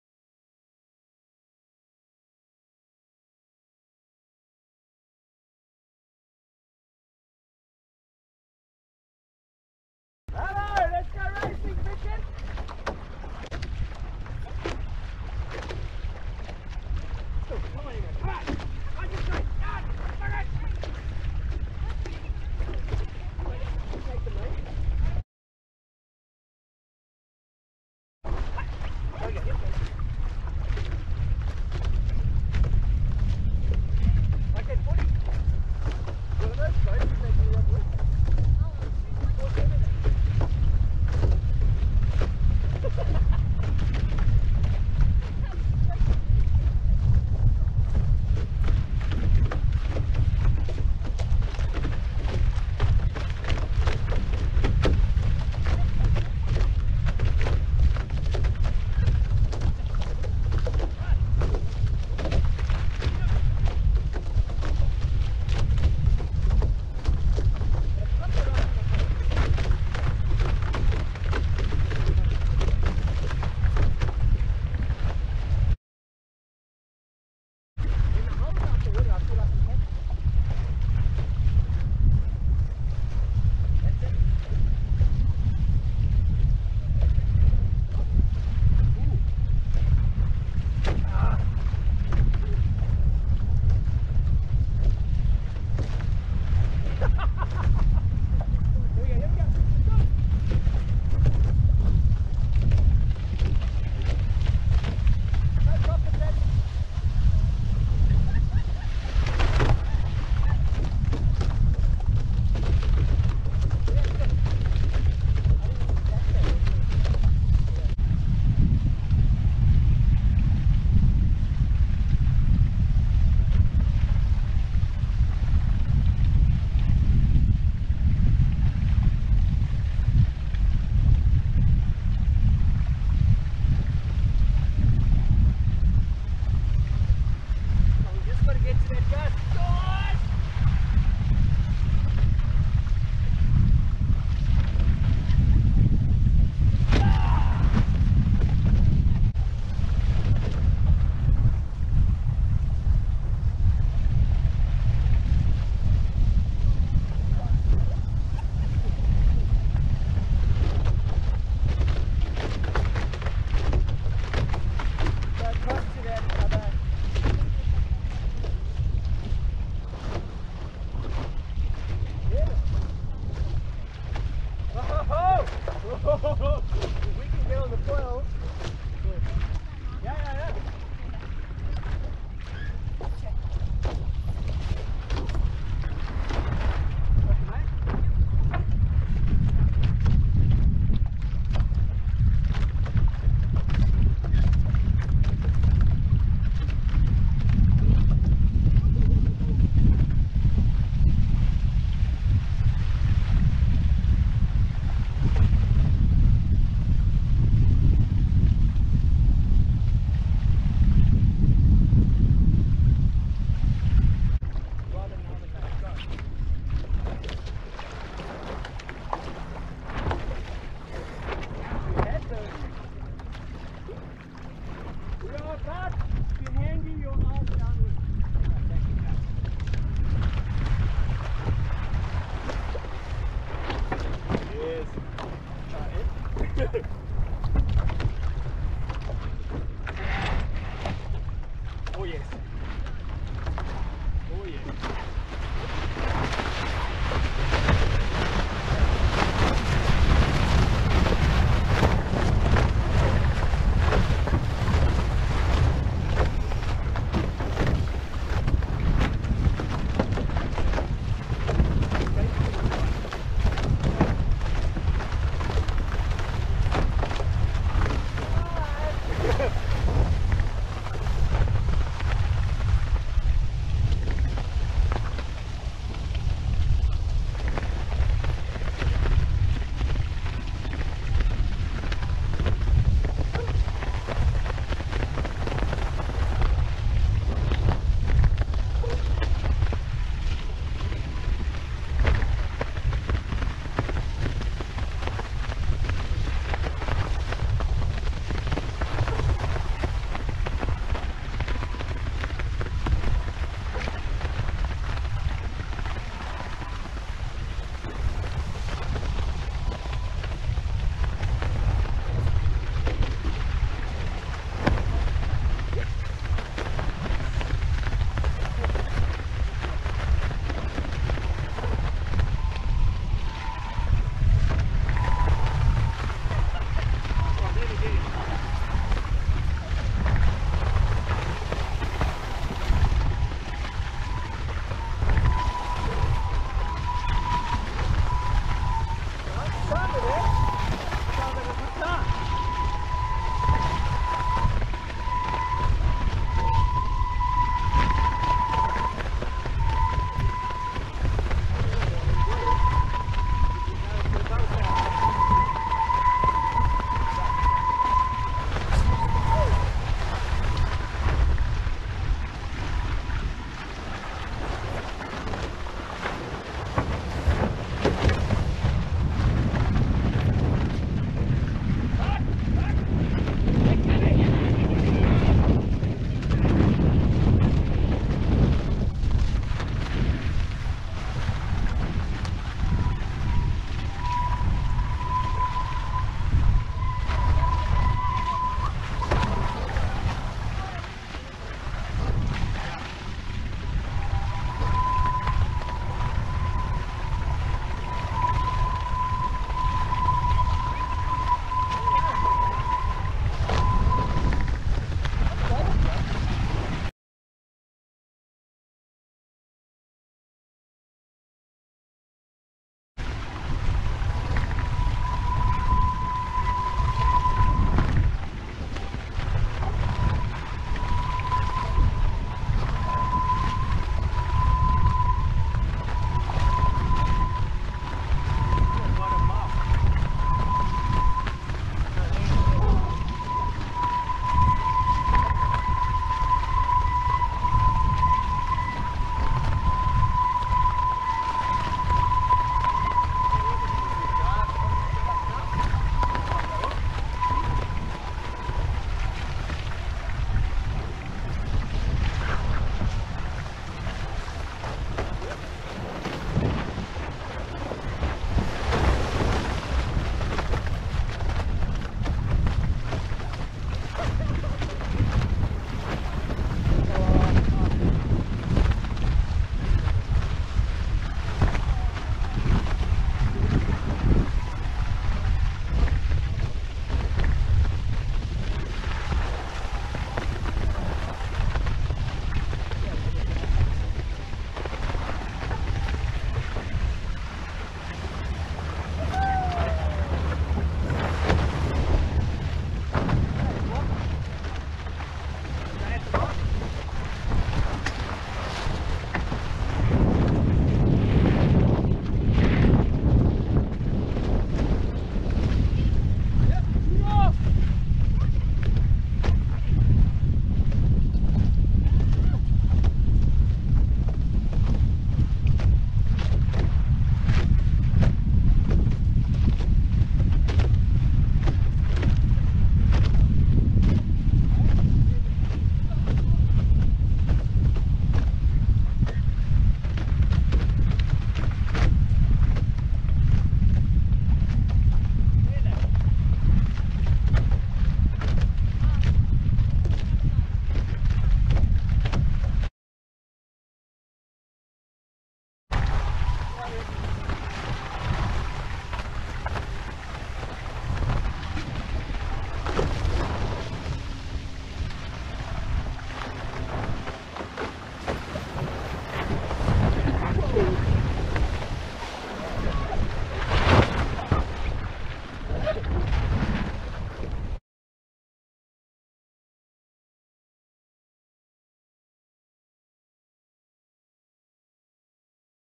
Lets go racing F16 National Champs on our Foiling Viper
We put the foiling Viper up against the fastest F16 in Australia. The breeze was a little patchy so foiling upwind was on and off. Watch the two teams put the boats head to head and have a bit of a laugh at some 470 light wind antics.There is a bit of sailor language, but its all in good fun.